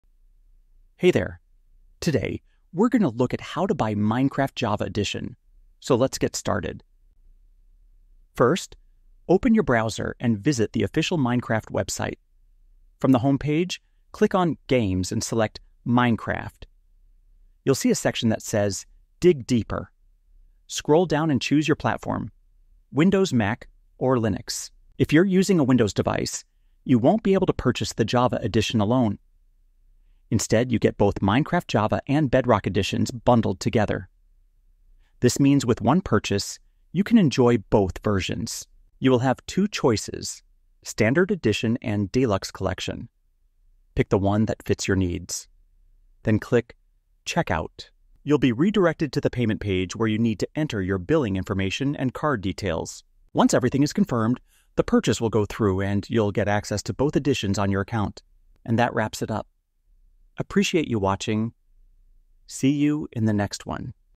How To Buy Minecraft Java Edition – Full Step-by-Step Purchase Guide (2025)
In this video, you'll learn How To Buy Minecraft Java Edition in 2025 with a full step-by-step guide. Whether you're new to Minecraft or switching from Bedrock, this tutorial shows you exactly How To Buy Minecraft Java Edition through the official Microsoft Store or Minecraft.net.

We’ll walk you through account setup, payment, and how to access your game after purchase.

🧱 In this guide, you’ll learn:

How To Buy Minecraft Java Edition on Windows and Mac

How to create a Microsoft account for Minecraft

Where to purchase the official Minecraft Java Edition safely

What to do after buying to start playing immediately